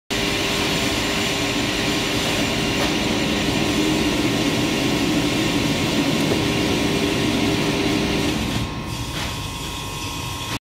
Gas burner Weishaupt WG30…/1-C ZM-LN error code 27H. Gasbrenner Weishaupt WG30…/1-C ZM-LN code 27H
Газовая горелка WG30…/1-C ZM-LN ошибка 27H. Отрыв пламени во время работы:

- Проверить настройки горелки;
- Проверить систему подачи топлива;
- Проверить датчик пламени.